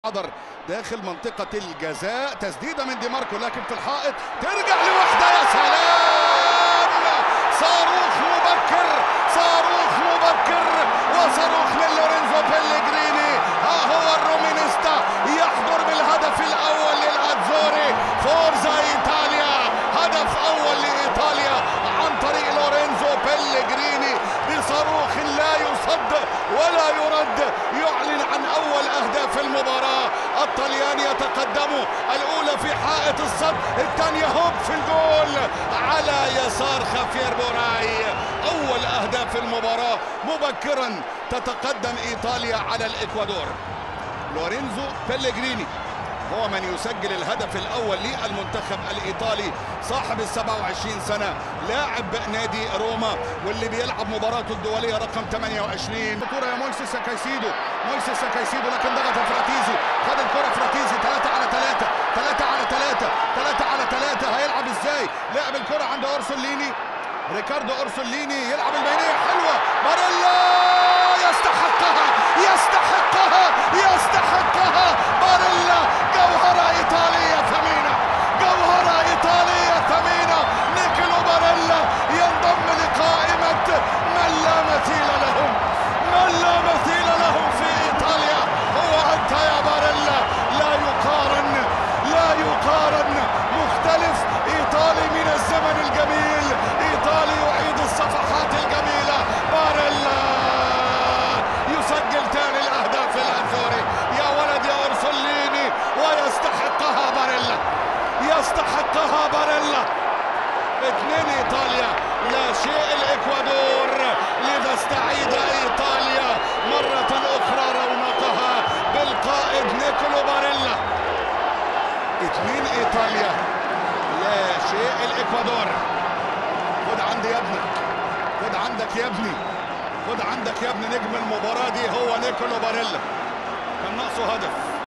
إيطاليا 2 - 0 الإكوادور | مباراة ودية
إيطاليا تفوز بهدفين دون رد على الإكوادور في مباراة ودية

شاهد هدفي المباراة

أبوظبي_الرياضية 1